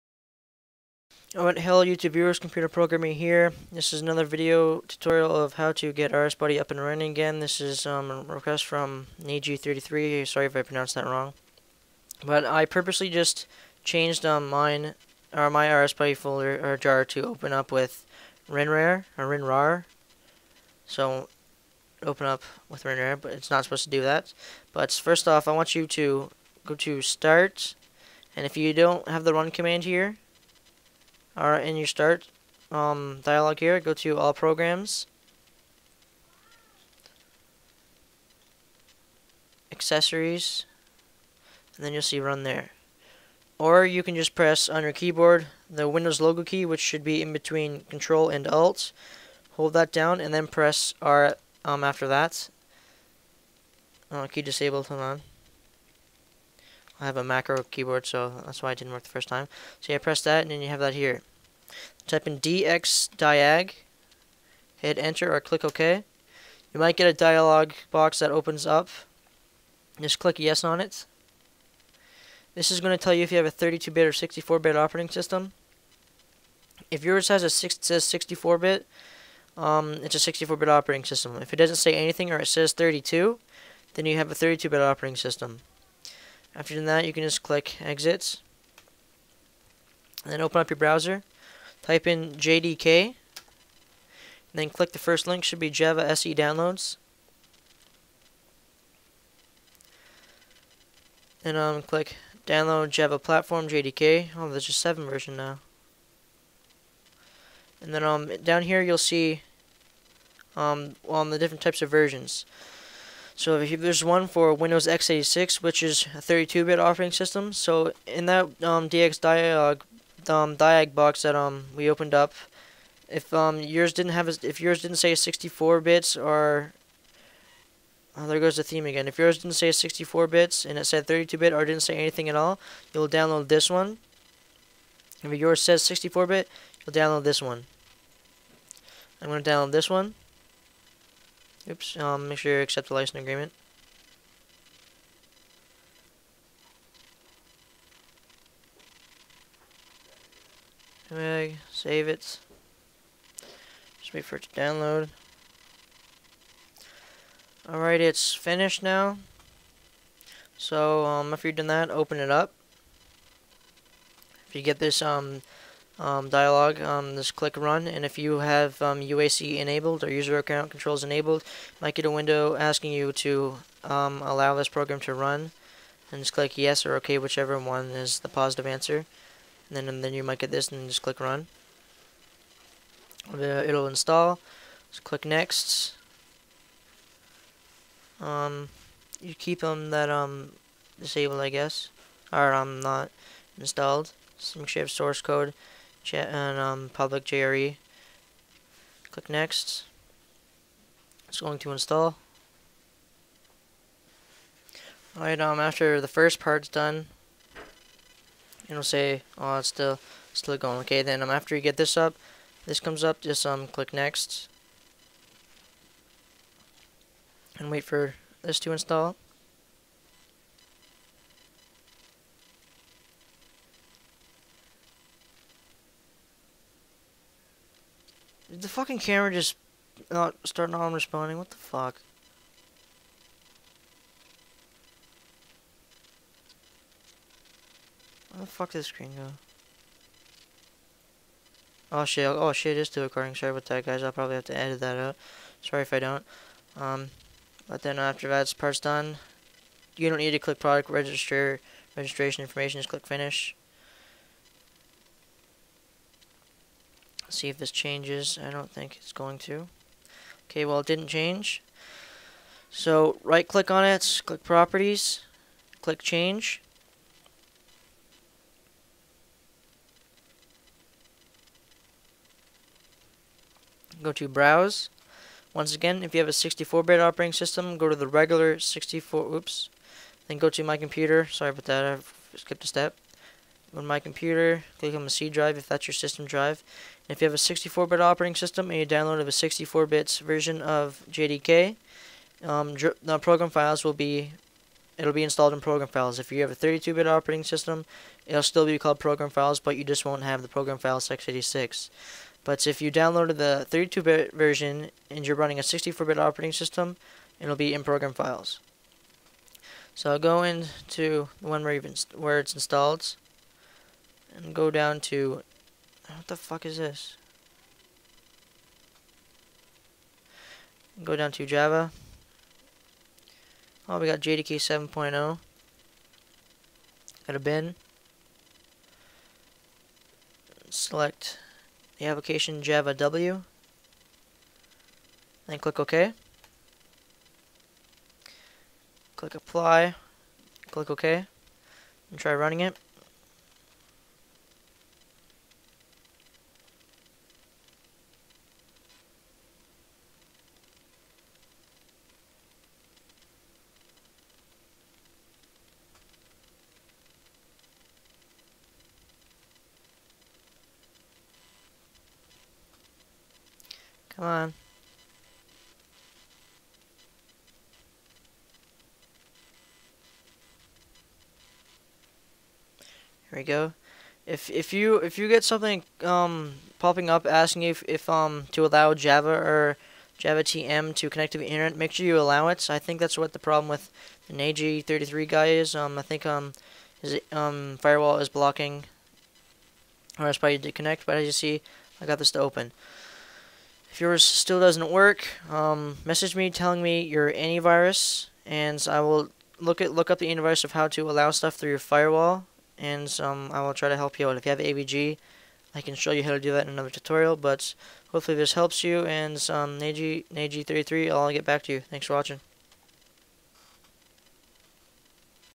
RSBuddy Problem 1 [FIX] - L3GiT
Sometimes when you download RSBuddy it might download with the wrong file extension such as a WinRAR file extension or if it does download as the right file extension which is .jar it might be set to open up with the wrong program such as WinRAR.

In this video I will show you how to download and install Java and select the correct program to open up RSBuddy with.

This is just one of many solutions of fixing RSBuddy and getting it to work.  Once I figure out on how to fix the reset of the problems I will post up a video.

CmptrPrgmr - L3GiT